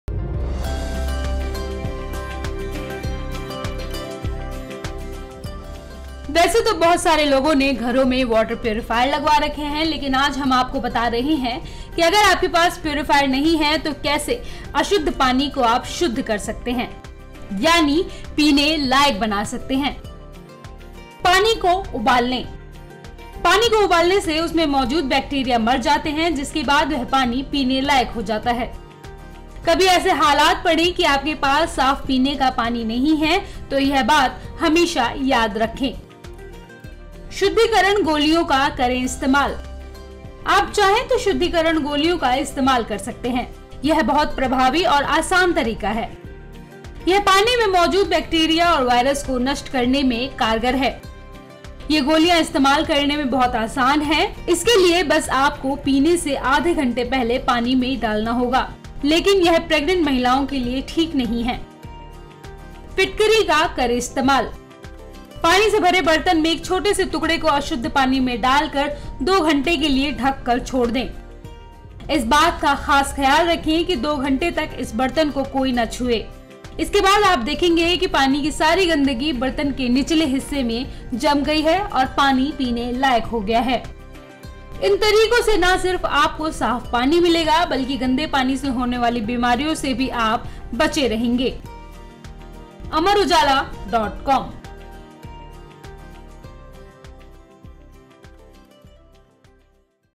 गंदे पानी को साफ करने के आसान तरीके | Amar Ujala
वैसे तो बहुत सारे लोगों ने घरों में वाटर प्यूरीफायर लगवा रखें हैं, लेकिन आज हम आपको बता रहे हैं कि अगर आपके पास प्यूरीफायर नहीं है तो कैसे आप अशुद्ध पानी को पीने लायक बना सकते हैं...